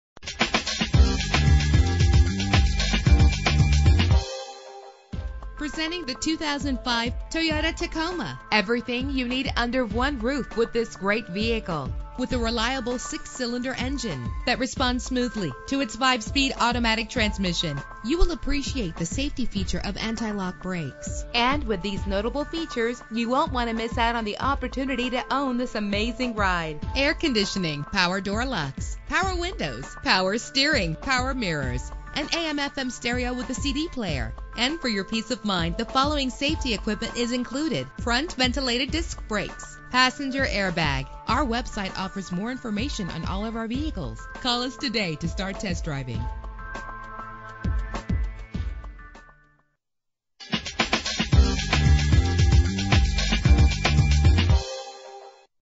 2005 Toyota Tacoma - Hollywood FL
Year: 2005
Make: Toyota
Model: Tacoma
Trim: PreRunner
Engine: 4.0 liter 6 cylinder 24 valve
Transmission: 5-Speed Automatic
Color: Blue
Mileage: 75987
Address:
1027 South State Rd 7